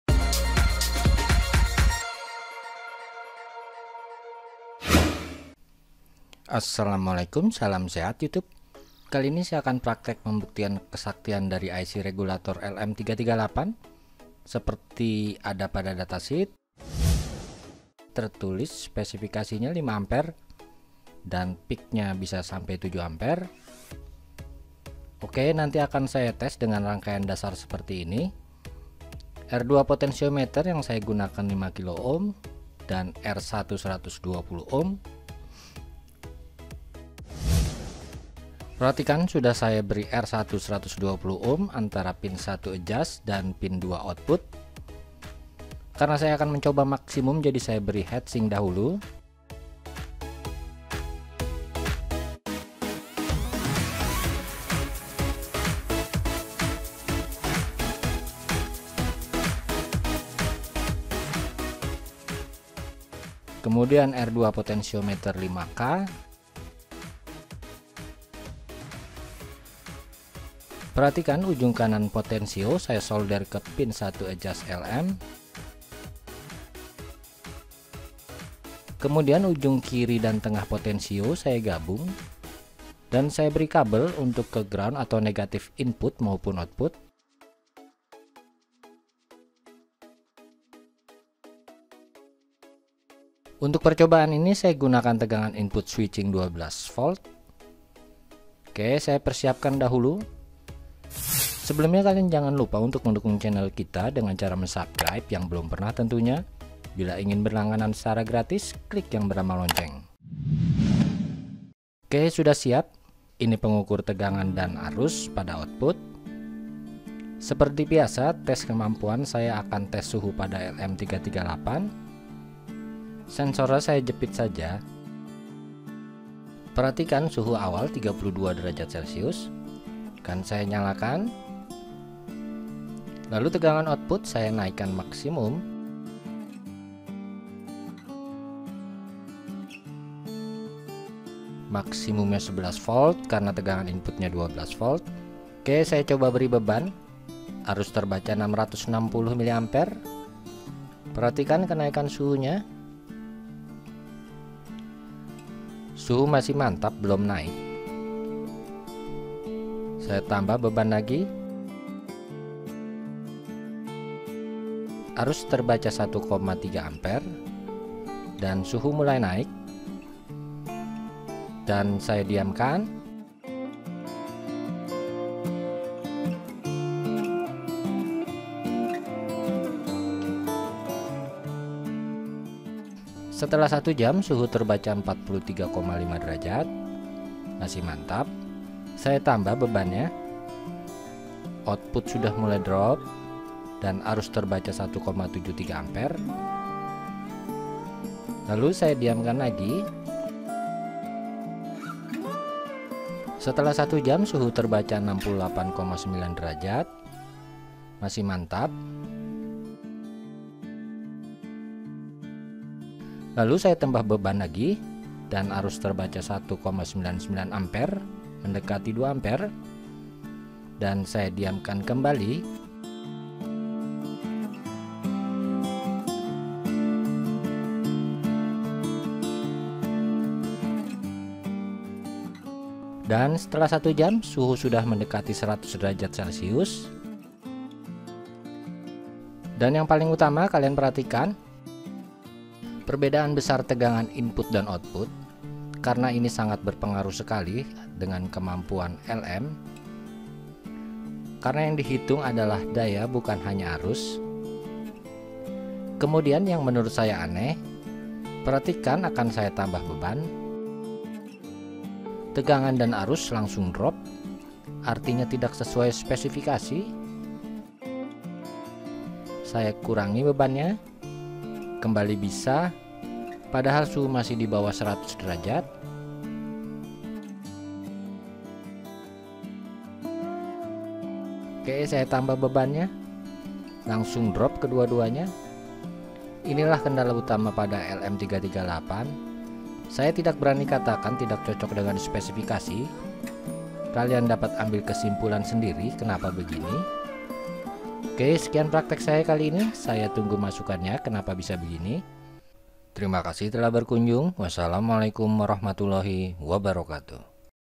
Cara Membuat Regulator Adjustable 5A LM338 adjustable power supply
Video ini menunjukan  atau test spesifikasi LM338 apakah sanggup 5A sesuai data sheet . Untuk melakukan ini, percobaan dilakukan dengan rangkaian standard yaitu menggunakan resistor 120 ohm dan potensiometer 5K ohm.
Hasil lengkapna dapat kalian lihat pada video.

00:00 Radal05
00:12 regulator LM338 Datasheet
00:40 Assembly
01:50 Test Regulator LM338